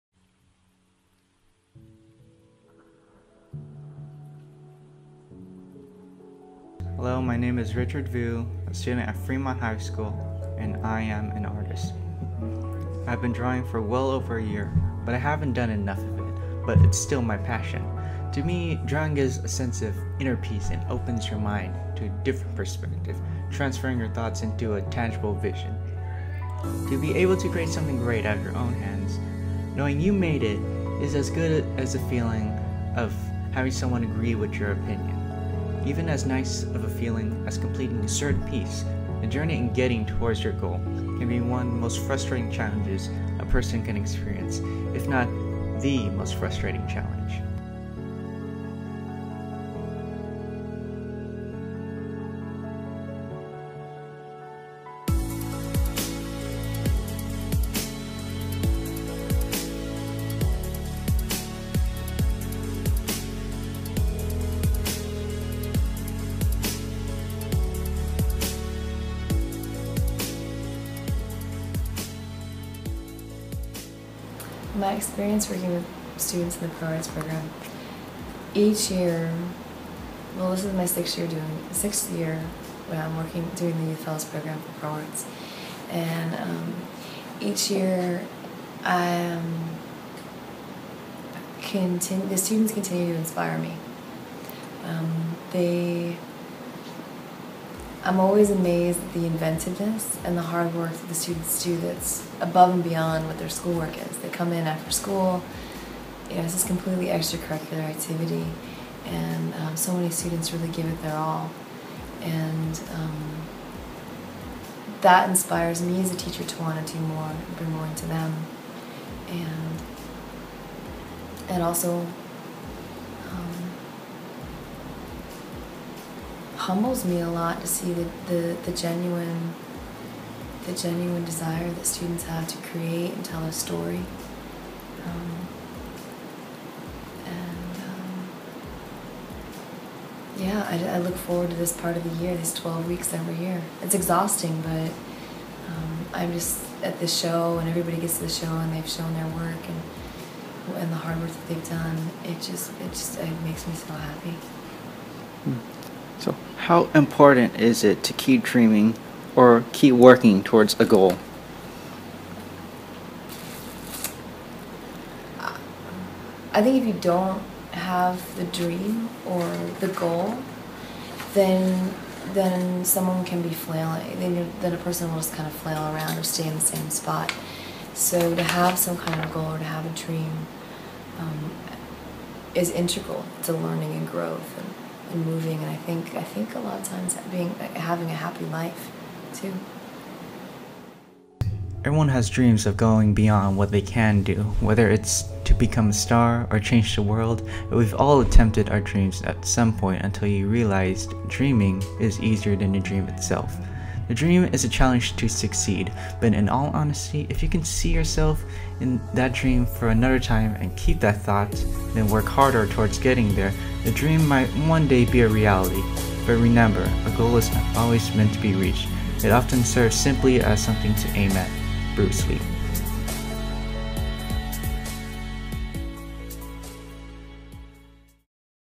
Artist by Richard Vu
Media Academy student Richard Vu owns his talent and enjoys cultivating his skills at the Pro Arts Gallery in Oakland, CA.